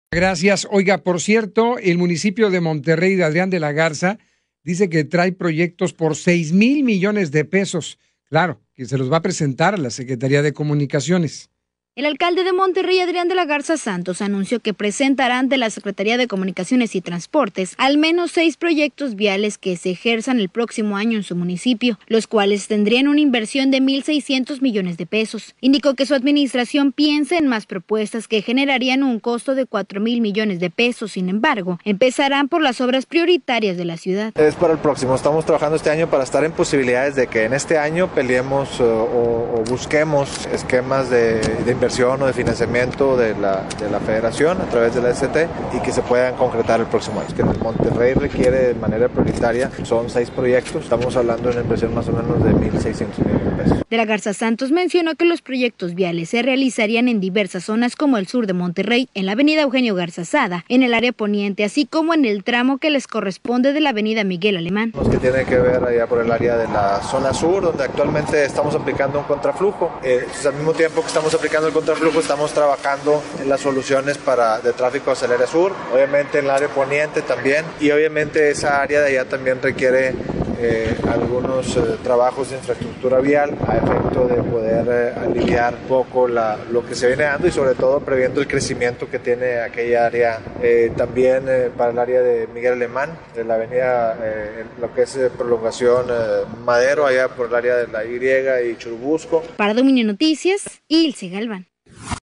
Dominio Noticias - Anuncia Adrián de la Garza proyectos viales en Monterrey
El alcalde de Monterrey, Adrián de la Garza Santos, anunció que presentará ante la Secretaría de Comunicaciones y Transportes al menos seis proyectos viales que se ejerzan el próximo año en su municipio.